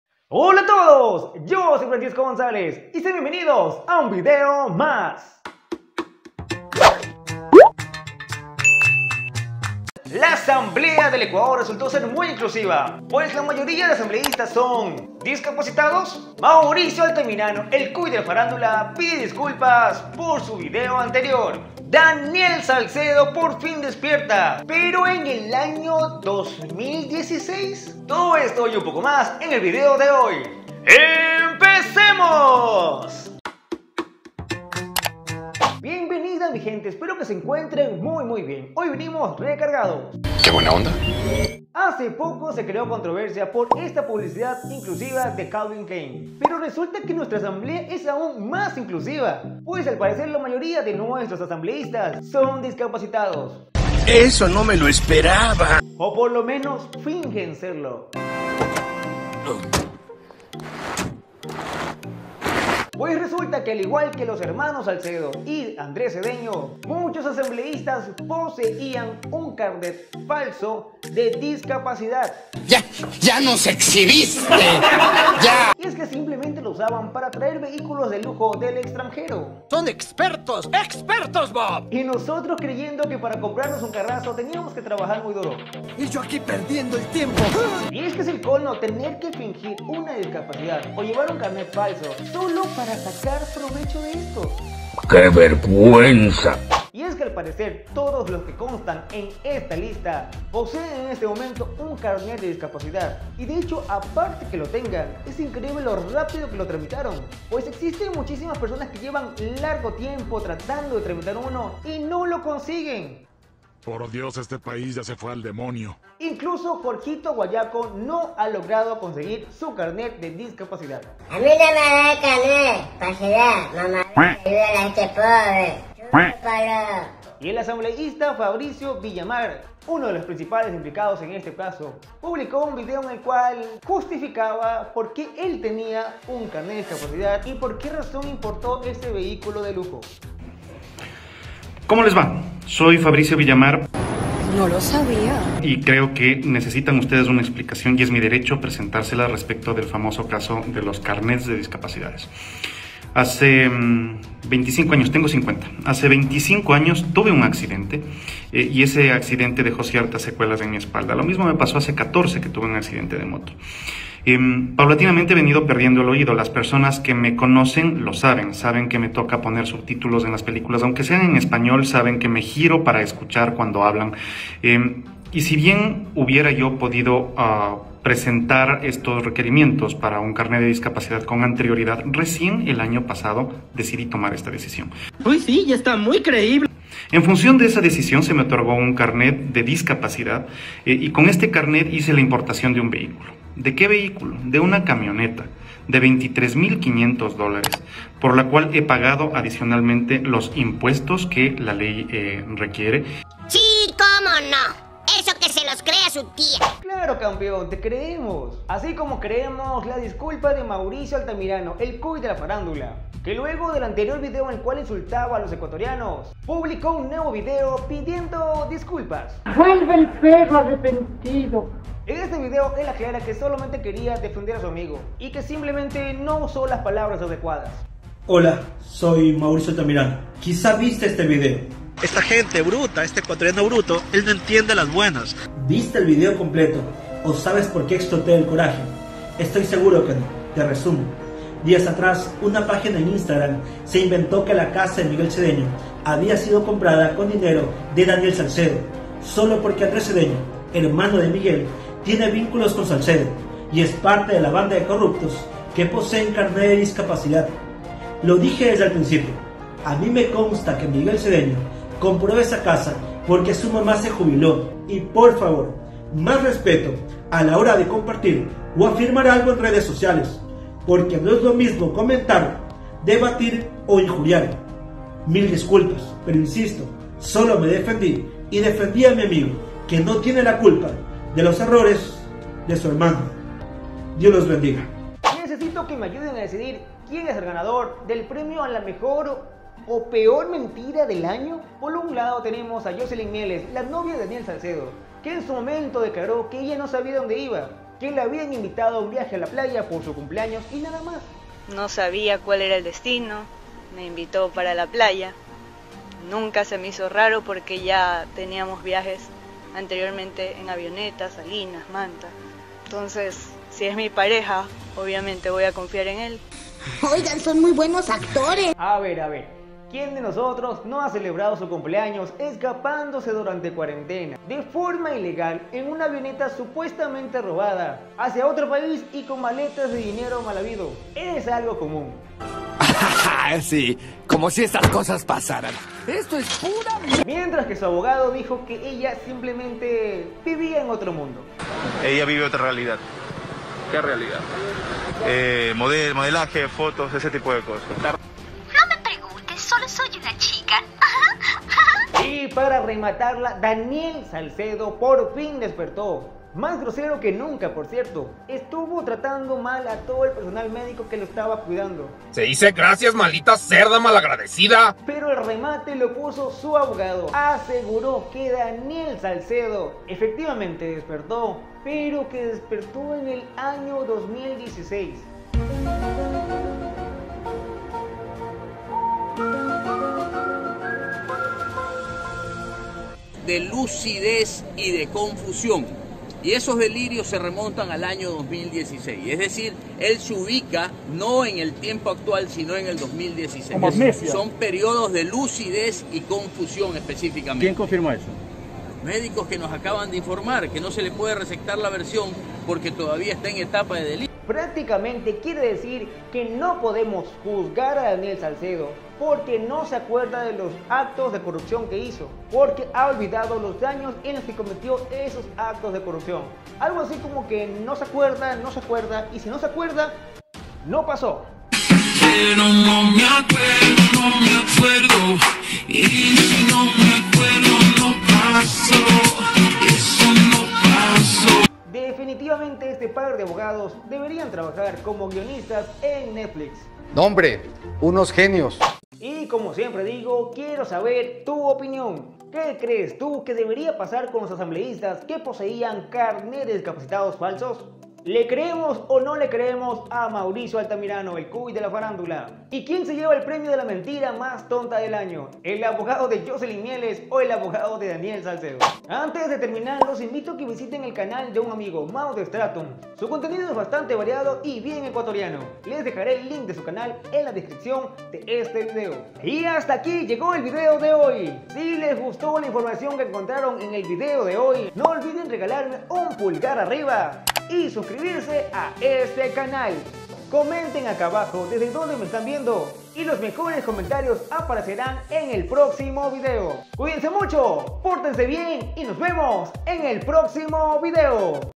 ASAMBLEÍSTAS ¿DISCAPACITADOS? Y DANIEL SALCEDO DESPIERTA ¿EN EL AÑO 2016?
Tenemos el caso de los asambleístas con carnet de discapacidad falsos, el Cuy pide disculpas a los ecuatorianos y Daniel Salcedo despierta ¿En el año 2016?

EL CANAL RECOMENDADO DE ESTA SEMANA ES ROCK N'  VLOGS